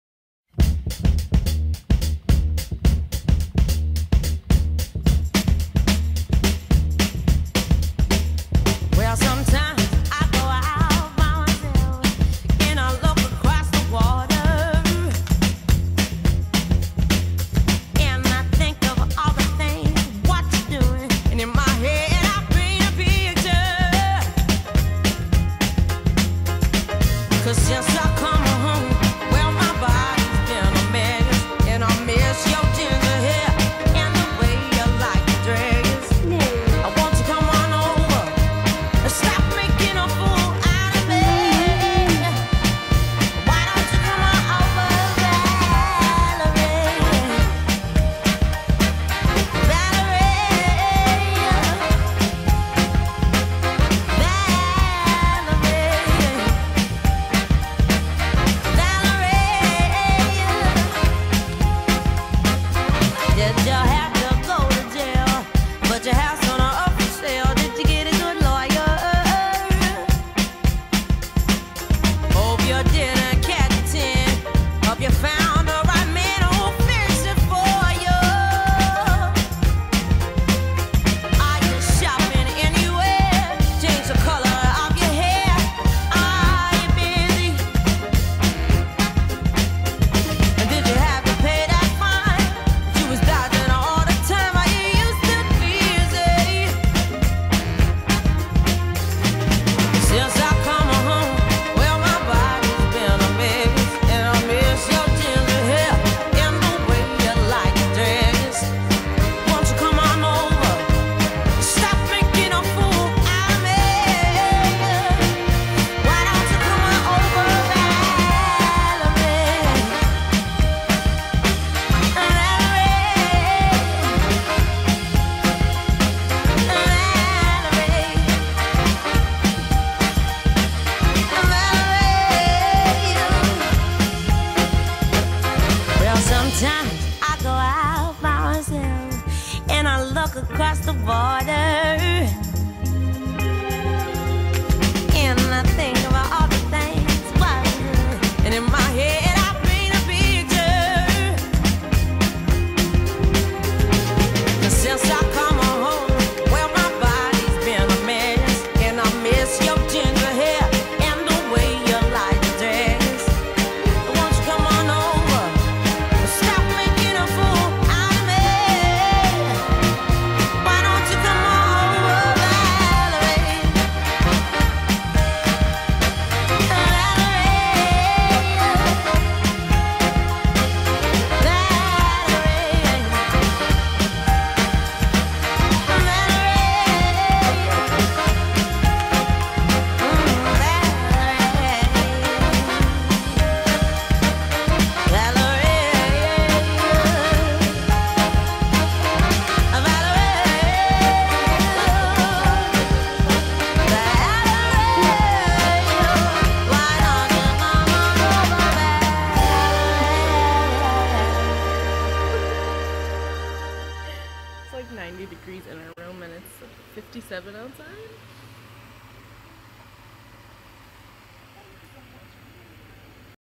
Afternoon Tea Bus around London
We flew into London our first day in the UK and took a delightful bus tour around the city and wandered the streets a bit afterward